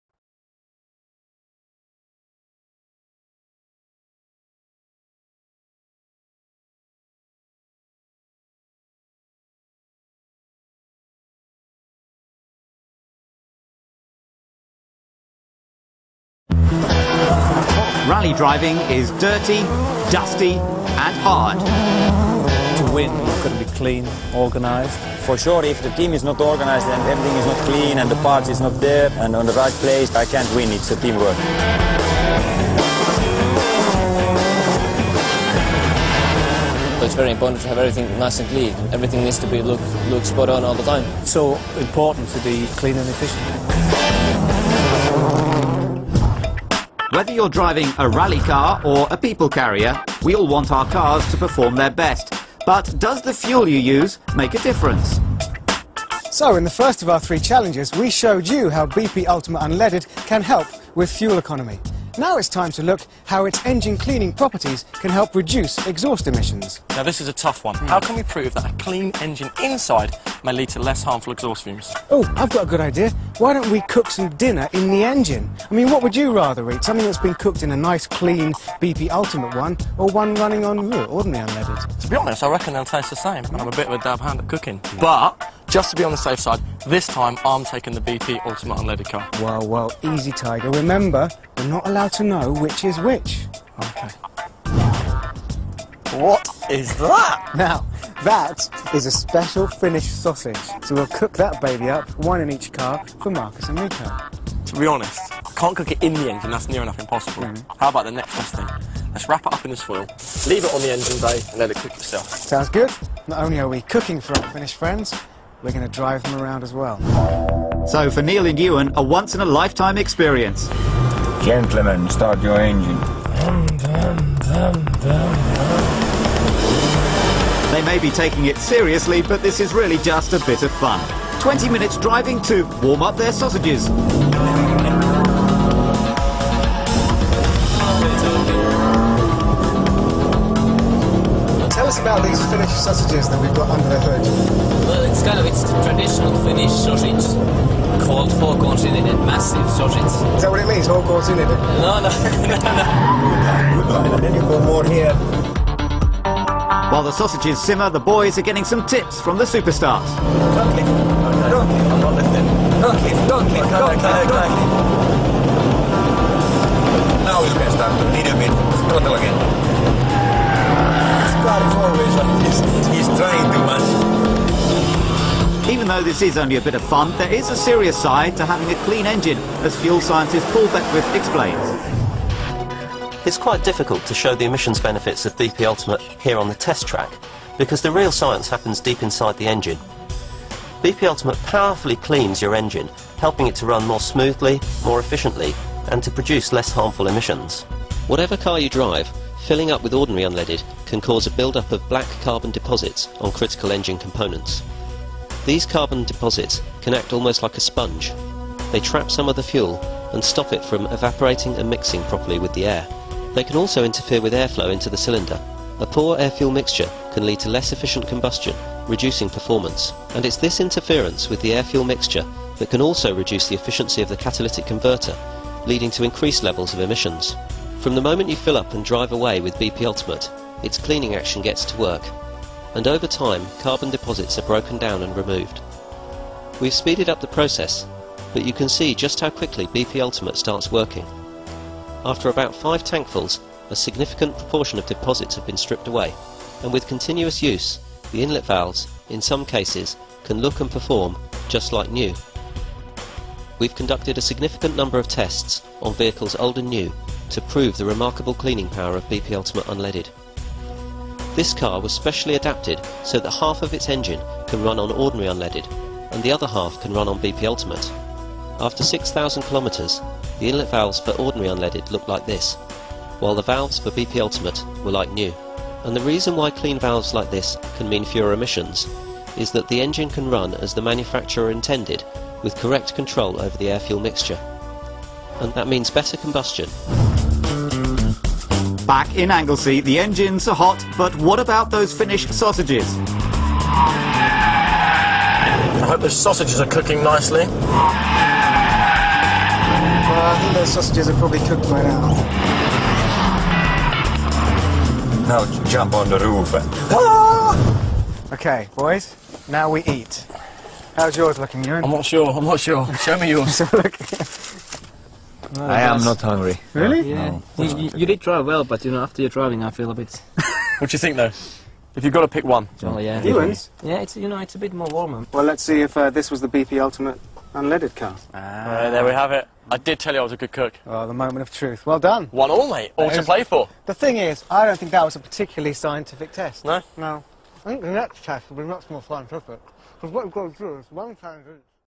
BP_Ultimate_Challenge_Test_2_Can_it_lower_emission
Sin depender de las necesidades de conducción y cualquiera que sea auto que manejas, el uso de gasolina sin plomo pueden causar una acumulación de negro de humo y acumulación de depósitos indeseados en los componentes críticos del motor. Estos depósitos de carbono pueden actuar casi como una esponja. Atrapan parte del combustible y evitar que se evapore y la mezcla sea la adecuada con el aire. También pueden interferir con el flujo de aire en el cilindro. Un aire de mala mezcla de combustible y puede dar lugar a menos de combustión eficiente, reduciendo el rendimiento y es esta la interferencia con la mezcla aire / combustible, que también puede reducir la eficiencia del convertidor catalítico que conduce a mayores niveles de emisiones. Desde el momento en que se llenan y en coche con BP Ultimate, sus "acciones de limpieza se pone a trabajar y sobre los depósitos a plazo del carbono se rompen y se retira. Hemos acelerado el proceso, pero se puede ver la rapidez con BP Ultimate comienza a trabajar. Después de unos 5 tanques de relleno de una proporción significativa de los depósitos se han despojado de distancia y con el uso continuo también de las válvulas de entrada, en algunos casos, puede buscar y llevar a cabo como nuevo. Después de llevar a cabo un número significativo de pruebas en los vehículos antiguos y nuevos para demostrar el extraordinario poder de limpieza de BP Ultimate Gasolina esta se cumple en todos los casos. Otro experimento exigente fue adaptar un vehículo para que la mitad de su "motor puede funcionar con gasolina sin plomo corriente y la otra mitad se puede ejecutar en BP Ultimate, mas tarde tras recorrer unos 6000 kilometros las válvulas de entrada se pudo observar residuos de carbono en los asientos y en las mismas, mientras que en los dos cilindros alimentados con BP Ultimate sin plomo estaban como nuevos, lo que va a significar directamente menos emisiones contaminantes como cuando motor era nuevo, con un correcto control sobre el aire / combustible mezcla. Esto significa una mejor combustión siempre dará lugar a menos emisiones.

English:"Without depending on driving needs and whatever car you drive, the use of unleaded gasoline can cause a buildup of carbon black and accumulation of unwanted deposits on critical engine components. These carbon deposits can act almost like a sponge. Trap some of the fuel and prevent evaporation and the mixture is adequate with air. They can also interfere with the flow of air into the cylinder. A bad air / fuel mixture can lead to less efficient combustion, reducing yields and this is the interference with the air / fuel mixture, which can also reduce the efficiency of catalytic converter leads to higher emission levels. From the time they fill up and drive with BP Ultimate and its "cleaning action gets to work and on the carbon deposits are broken and removed. We have accelerated the process, but you can see how quickly BP Ultimate starts working. After about 5 tanks filled with a significant proportion of the deposits have been stripped away and also the continued use of the inlet valves, in some cases, you can look and perform like new. After to carry out a significant number of tests on old and new vehicles to demonstrate the extraordinary cleaning power of BP Ultimate Unleaded this is true in all cases. Another experiment was to adapt a vehicle demanding to half of its "motor can operate on unleaded gasoline outlet and the other half can be run on BP Ultimate, later after about 6000 km both intake was observed carbon residue on the seats and the same, while in the two cylinders fed BP Ultimate unleaded were as new, which will mean fewer emissions directly as when the engine was new, with proper control over the air / fuel mixture. This means better combustion will always result in lower emissions."